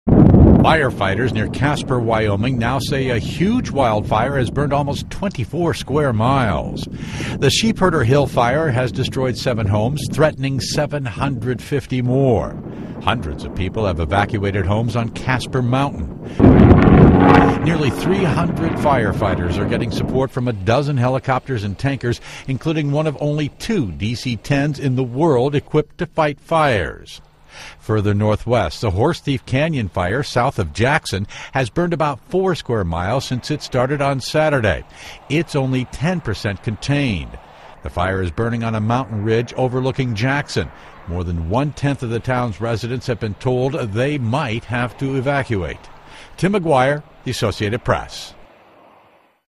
Wyoming Fire Burns 24 Square Miles
Wyoming's Casper Mountain fire has destroyed seven homes and burned 24 square miles. The Horsethief fire near Jackson Hole has burned four square miles. (Sept. 12)